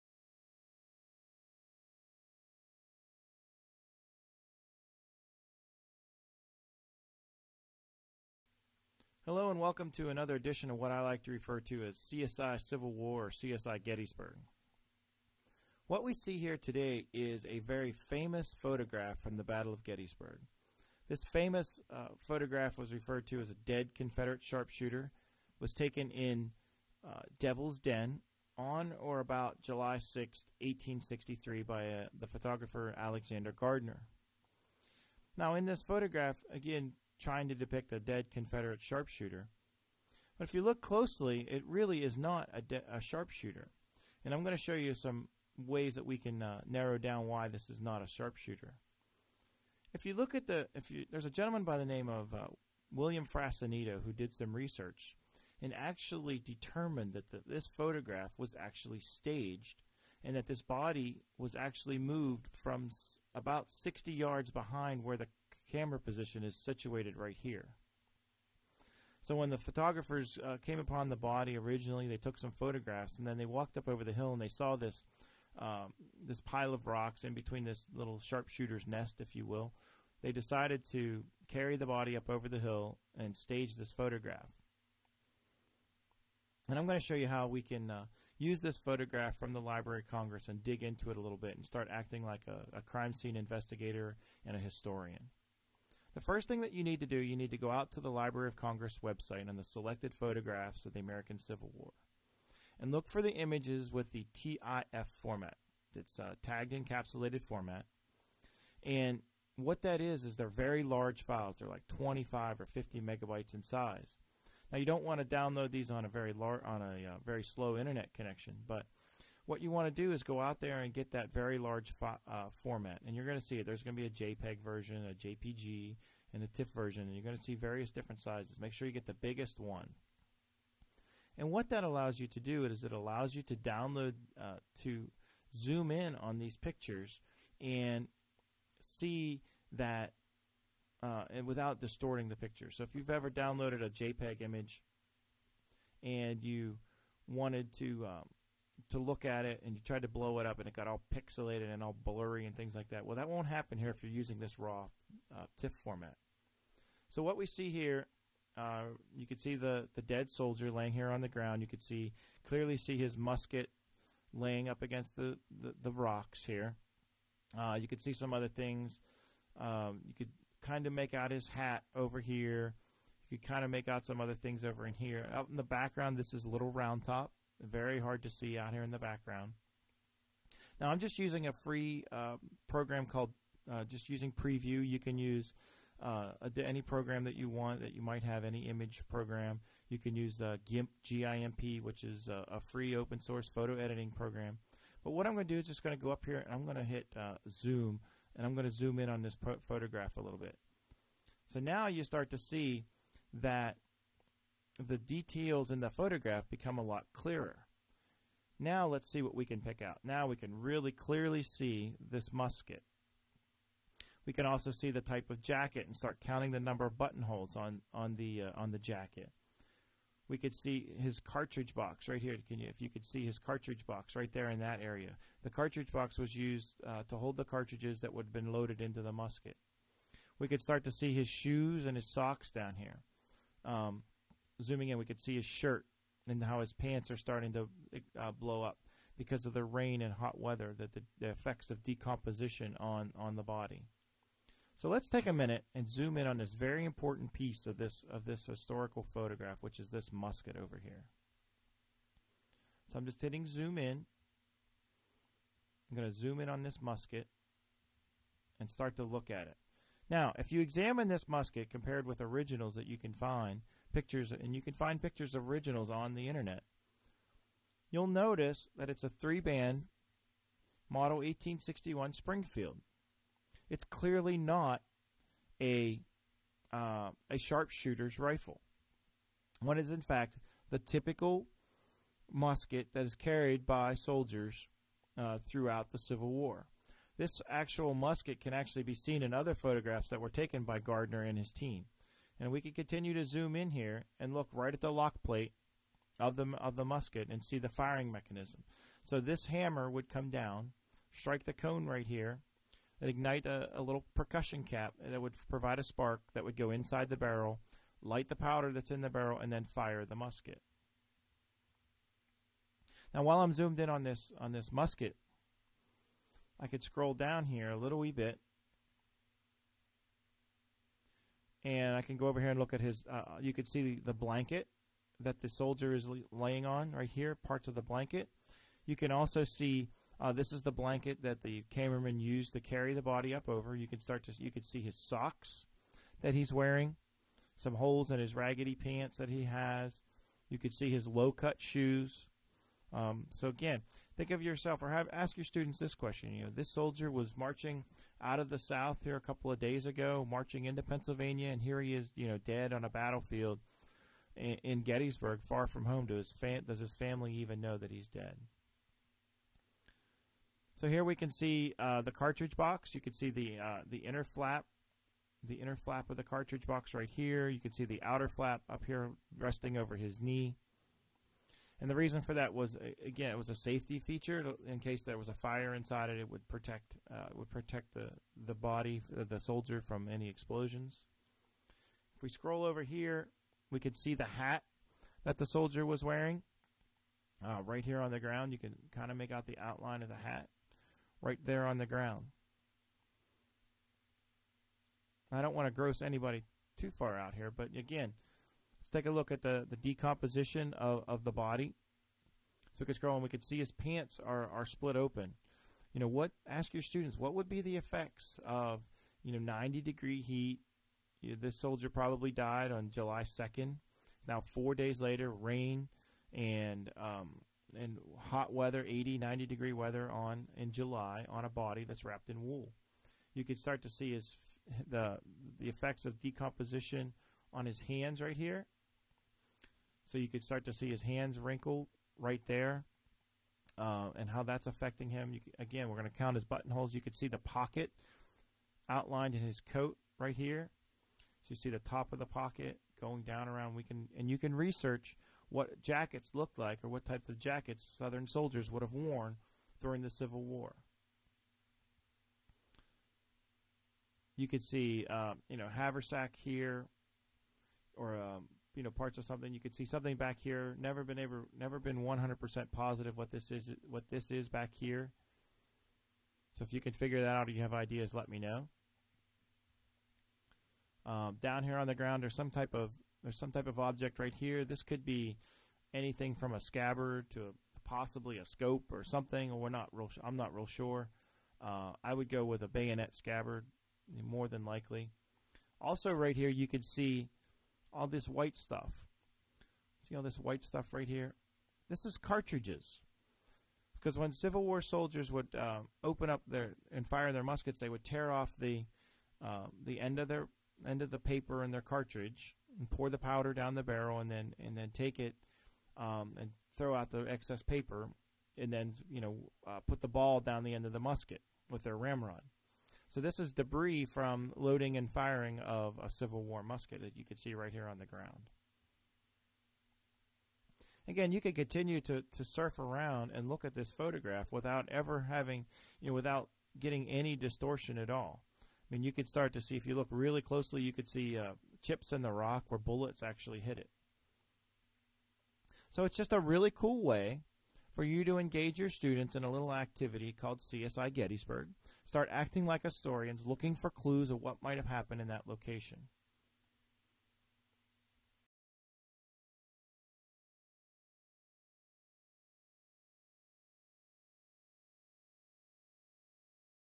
CSI - Gettysburg Sharpshooter
Learn how to use resources from the Library of Congress to get your students to act like historians.

This is the very famous Gettysburg photograph showing the famous Gettysburg Sharpshooter.